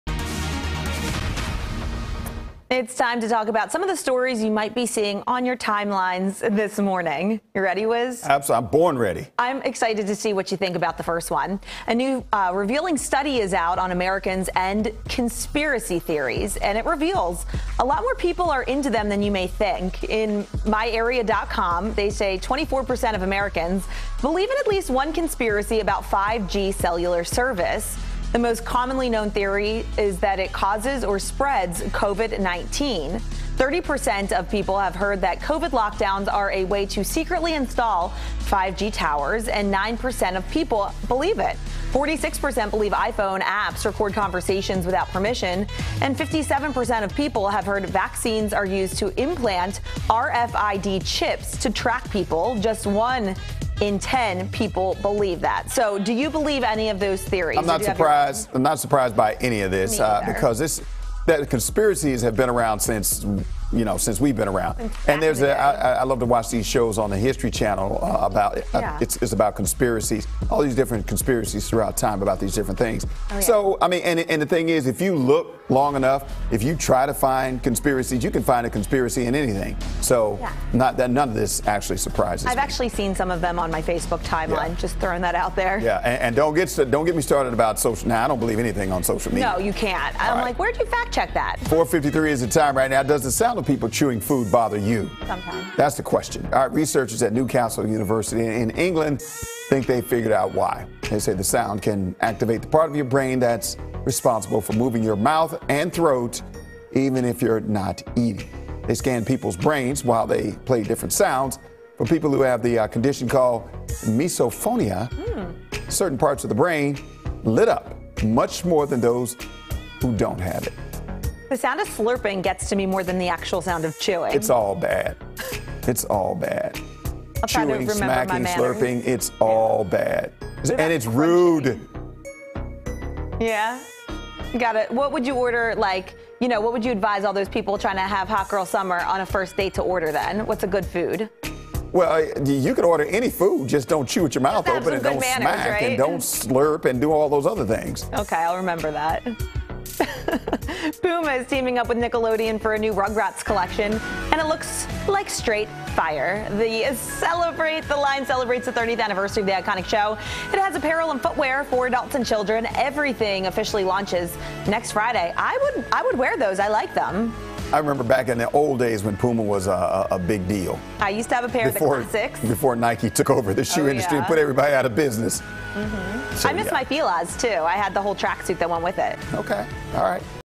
Nearly a quarter of Americans believe in at least one 5G conspiracy theory, study shows | FOX 5 DC
A new study reveals that 24% of Americans believe in at least one conspiracy theory centered on 5G cellular service.